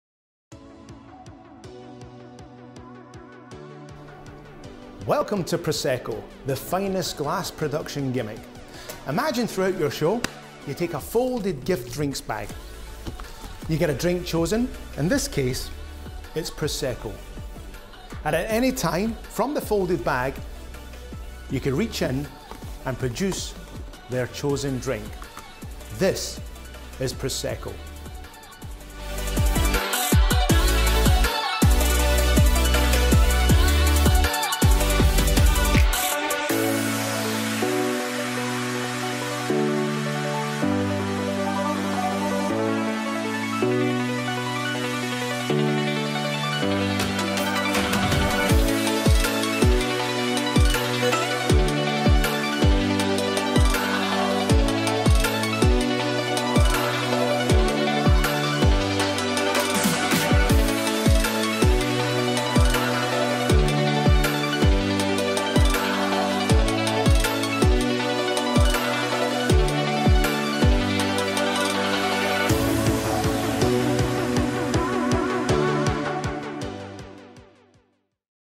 Pro Secco by Gary James
A production holder for flutes and small bottles. They truly seem to magically appear - looks great! It can also be used in reverse to vanish a glass. 100% no fear of the glass popping out, so you can relax and get on with your performance.